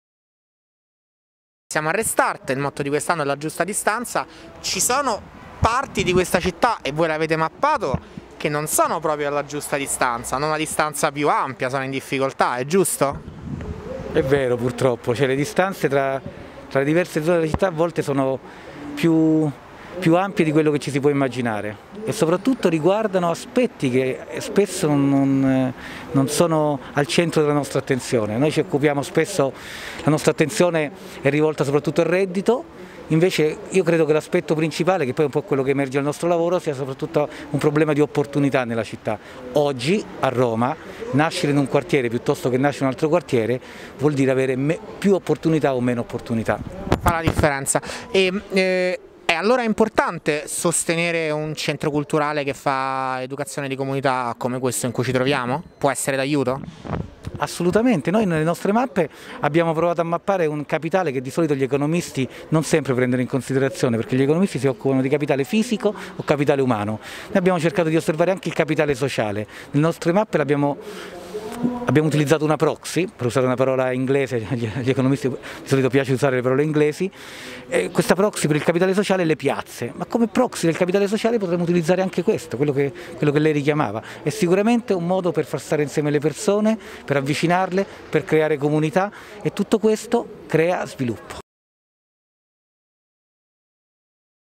RestartFest2020 - Intervista a Salvatore Monni
Intervista all'economista, autore de "Le mappe delle disuguaglianze", a margine della seconda giornata della sesta edizione di "Restart - Festival delle creatività antimafia e dei diritti / La giusta distanza" (8-9-10 ottobre, Roma)